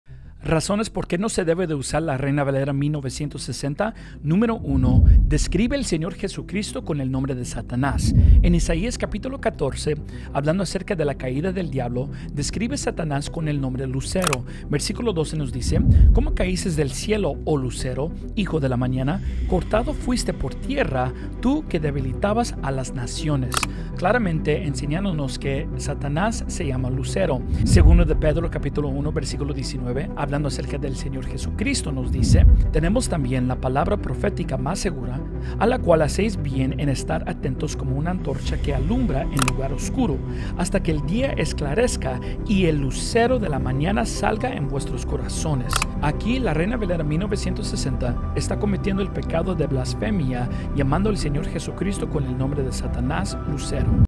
😈 JESÚS = SATANÁS según la REINA VALERA 1960! 💔 (LEER COMENTARIO FIJADO)
🚨 La biblia RV1960 comete el pecado de BLASFEMIA! ⚠️ Por lo tanto esta traducción del 1960 es corrupta. La Reina Valera 1960 es una edición que posee muchos errores intencionales y no debemos utilizar esta falsa versión de la biblia. En este video verás UNO de los tantos errores y blasfemias cometidos por los traductores de esta edición, está claro quien es el lucero de la mañana (lea el primer comentario fijado de este video) y este tipo de error no es tolerable. Entiendo que la Reina Valera 1960 es posiblemente la versión mas utilizada hoy en día, entonces aquí va la pregunta:

👉 ¿Qué Biblia utilizar en español? En mi caso, utilizo la Reina Valera Gomez 2010 (RVG2010) pero existen otras versiones fieles en su texto y mayormente correctas según el texto original, como la "Reina Valera Purificada", la "Biblia del Oso", la "Reina Valera Antigua 1602 (Bíblia del Cántaro)" e inclusive la Reina Valera 1865 es mejor que la Reina Valera 1960.

🟢 Si tienes dudas sobre la salvación o aun no eres salvo, vea este video:

♦ Biblia usada: Reina Valera Gomez 2010. Vea este video para saber por qué no usar la Reina Valera 1960/95